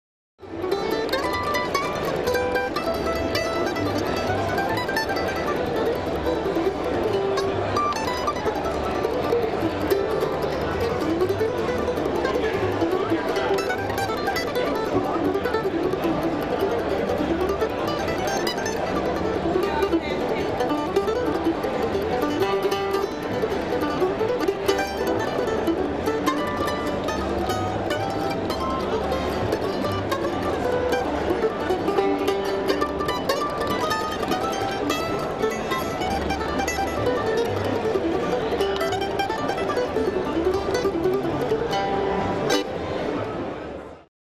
Dave Harvey plays The Orville Gibson Mandolin
Dave cuts down on the new exclusive one-off mandolin at the IBMA convention, 2008